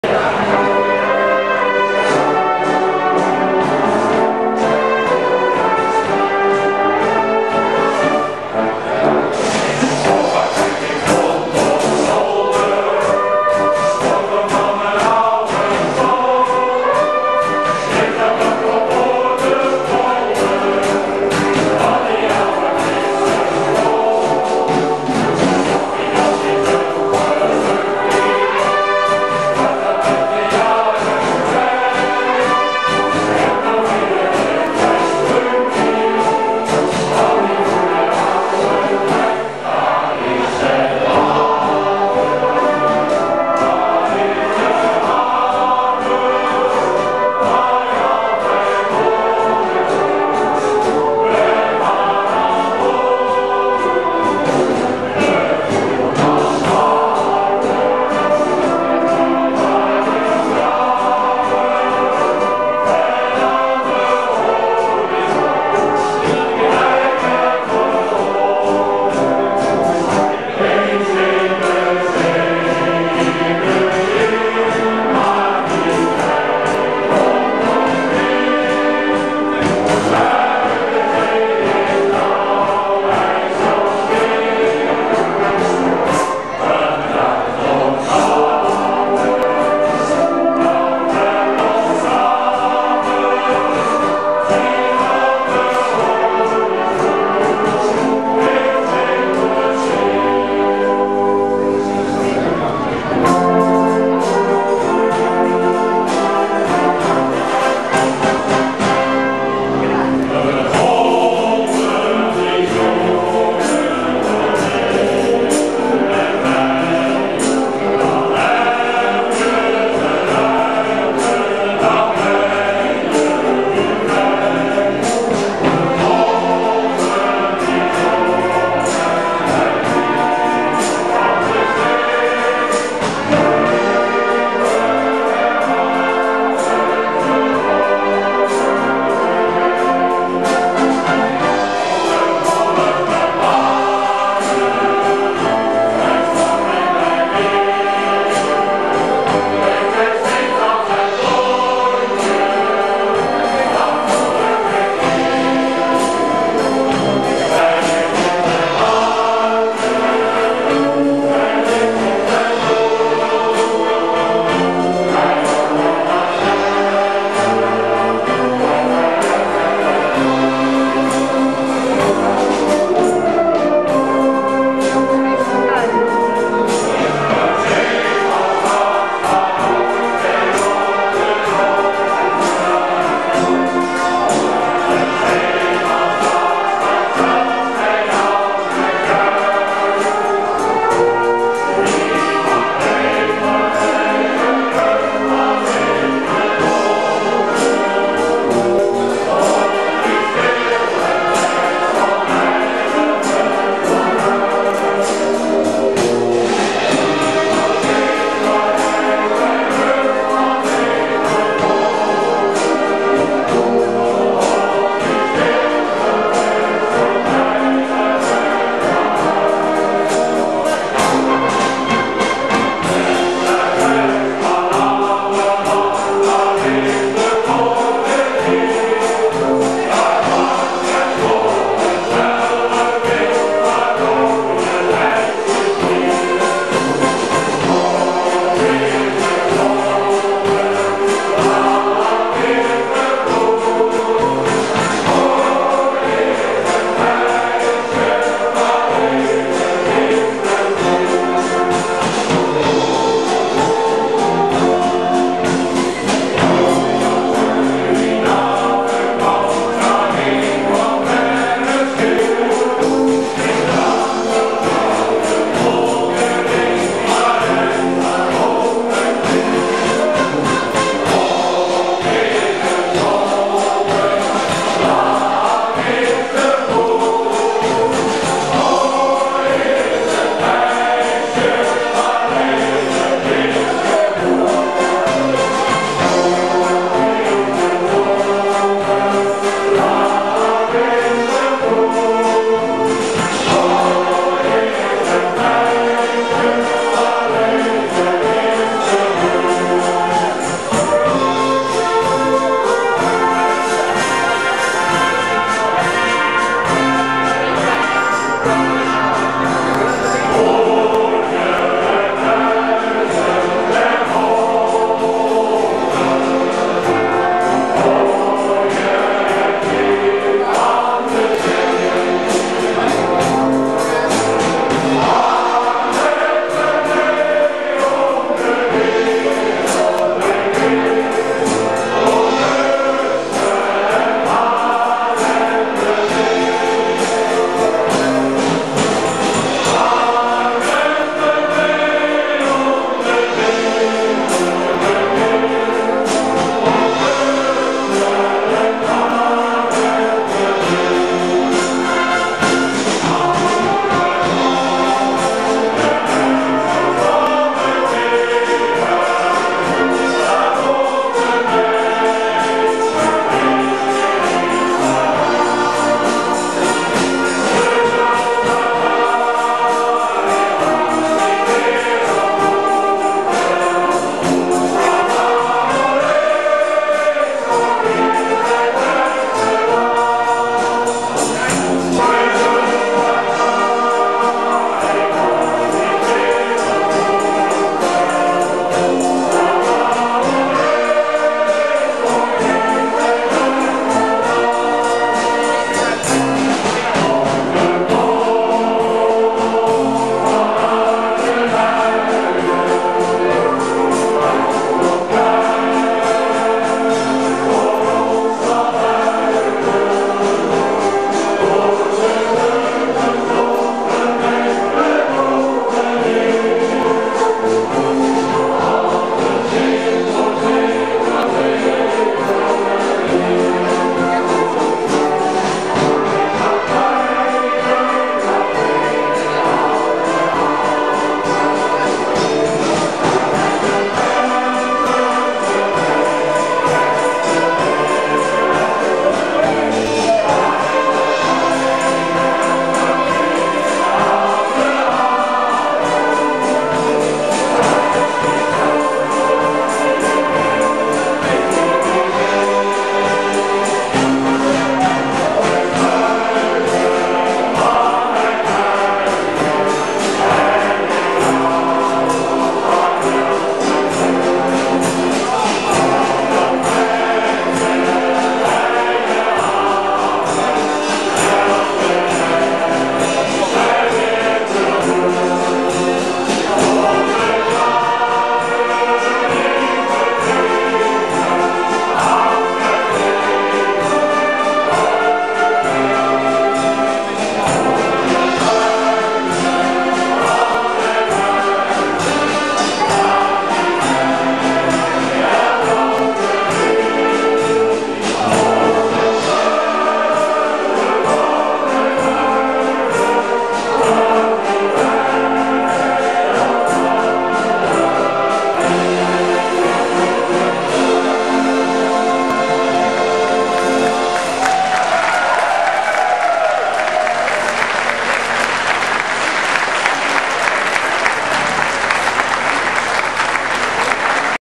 zeemansliederen medley
Optreden tijdens Koffieconcert op de Marker Havenfeesten 2010.
Diverse Zeemansliederen, in samenwerking met het Marker Mannenkoor

Gefilmd door Jan Cornelis Zeeman